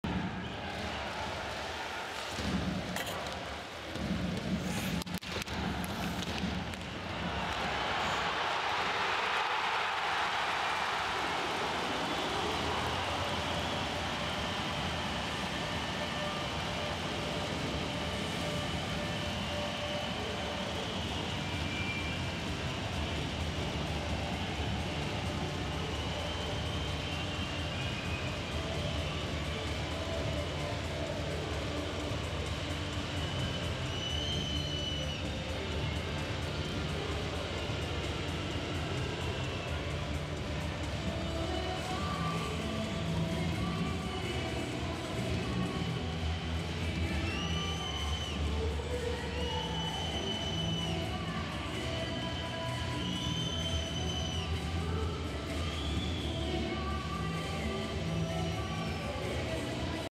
David Booth first KHL goal with Avangard
Avangard Omsk newcomer David Booth sends it home from the doorstep